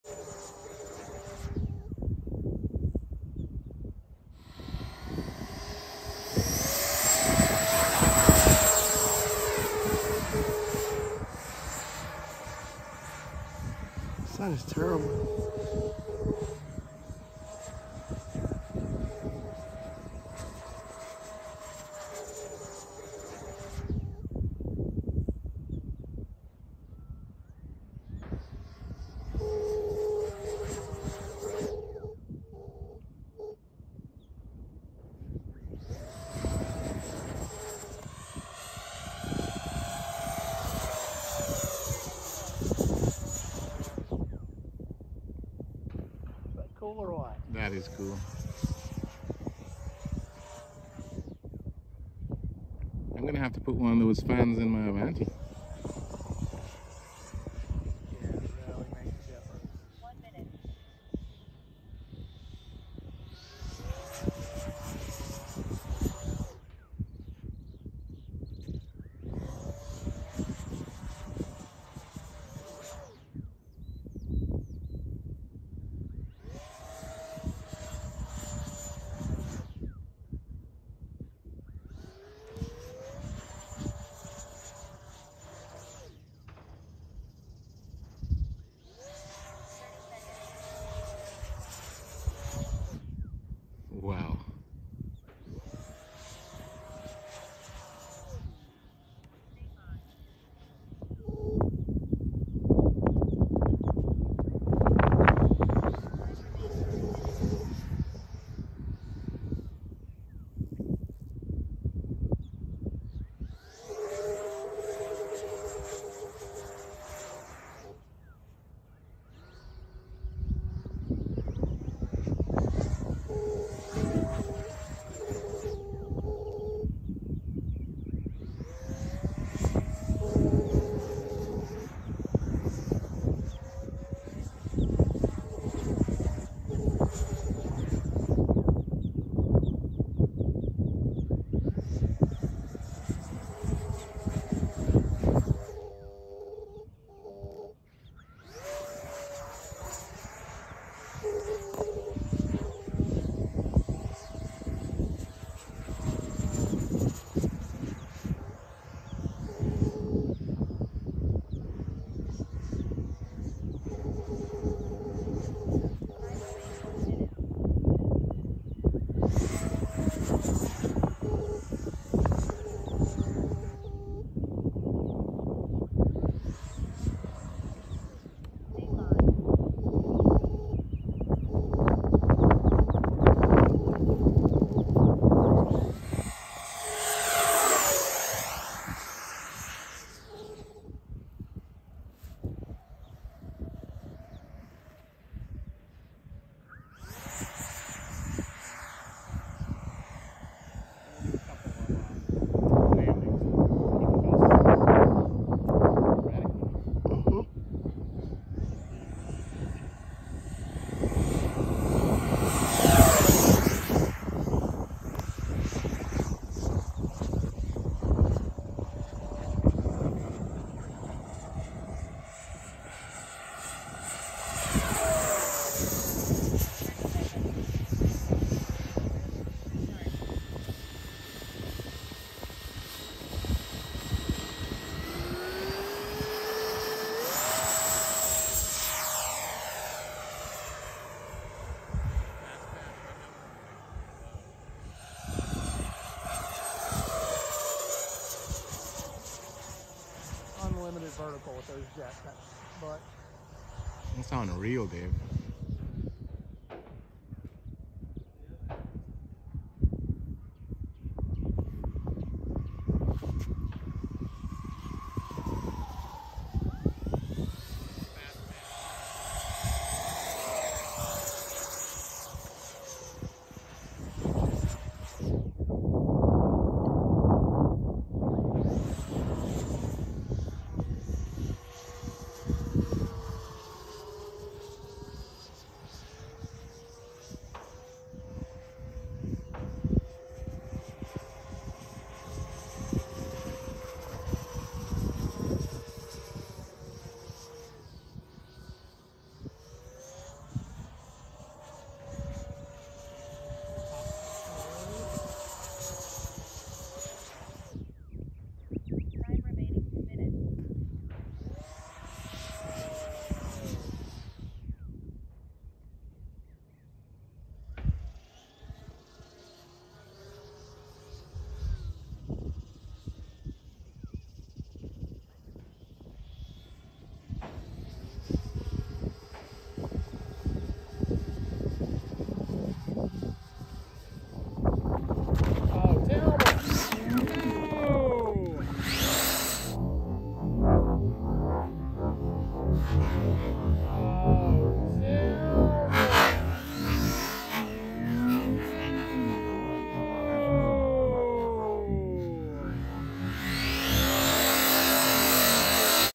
Extreme Jet Aerobatics & Speed with Crash!
Thanks to Pilots Alex and Bruce for some great video work. Jet will be quickly repaired and back in the air.